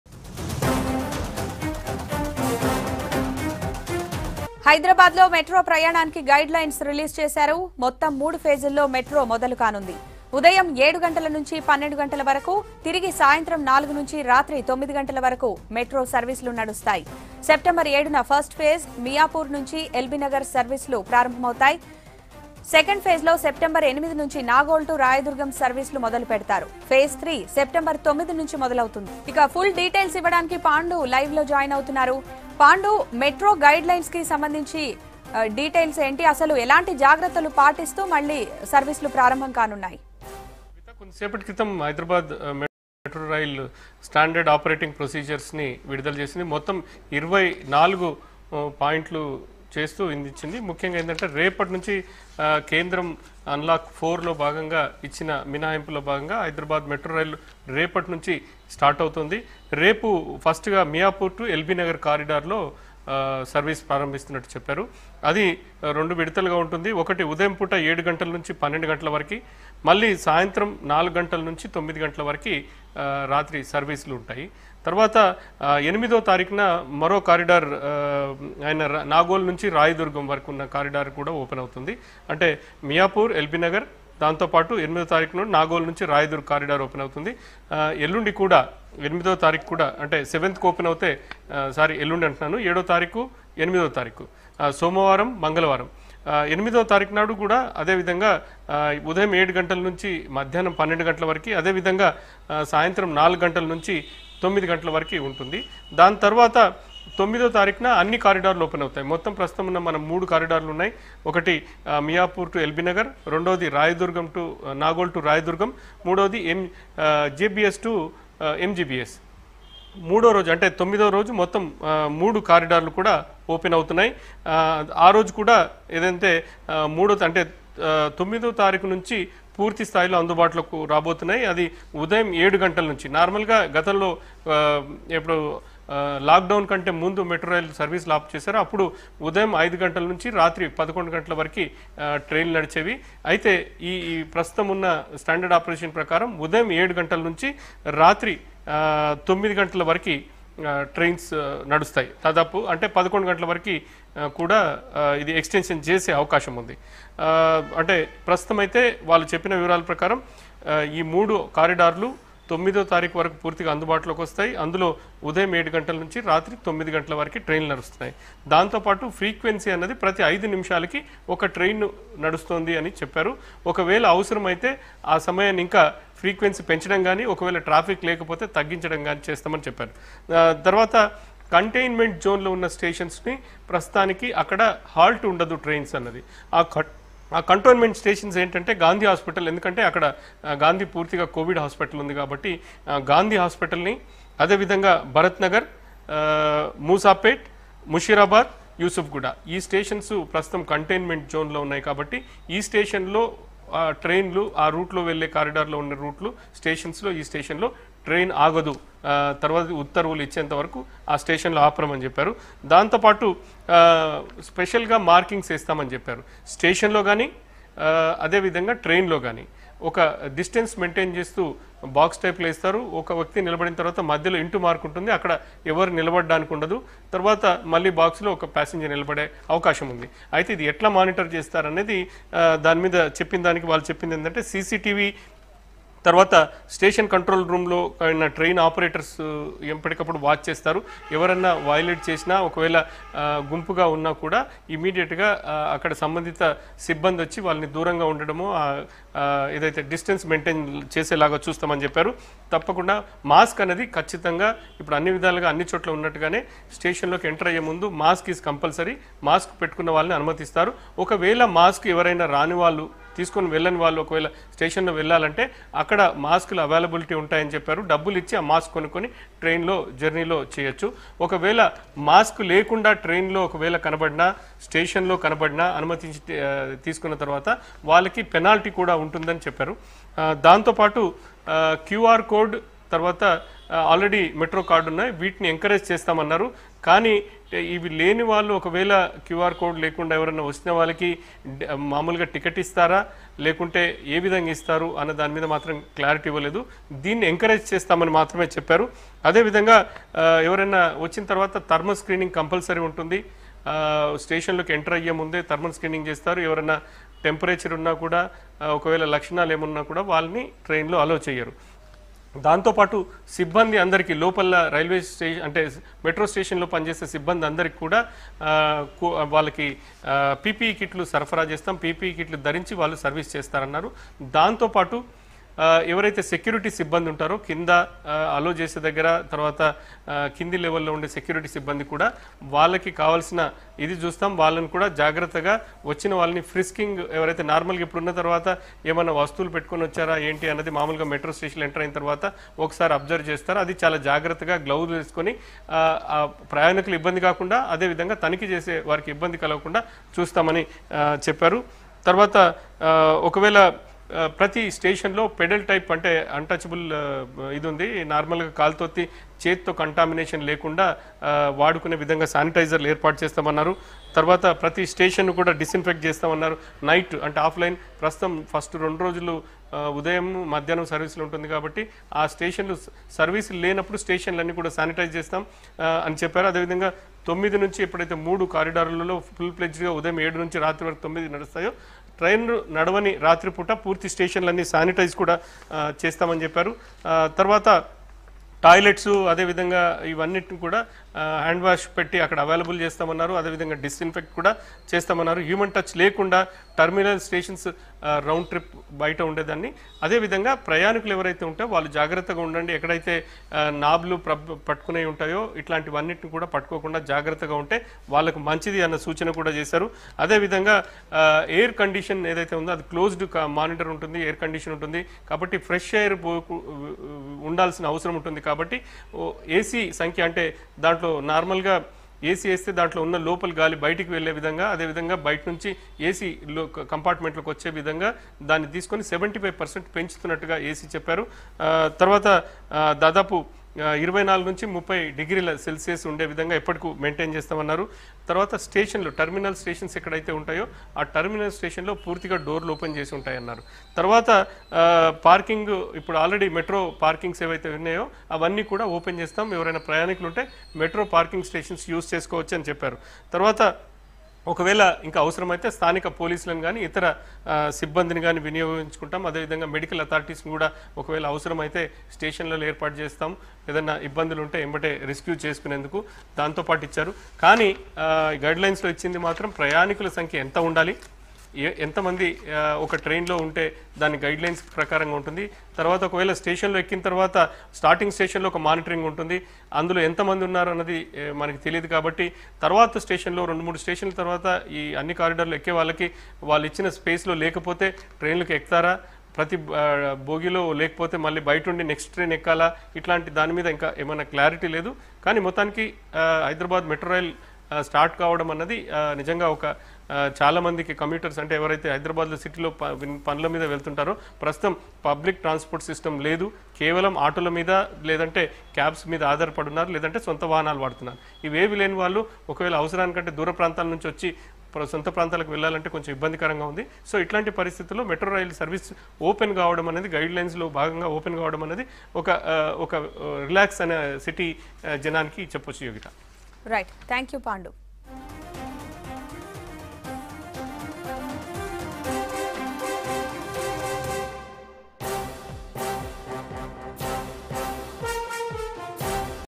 Hyderabad Metro Announces New Guidelines | V6 News
The Hyderabad Metro Rail Limited (HMRL) is all set to resume its services for public from September 7,And released new guidelines for running metro rails.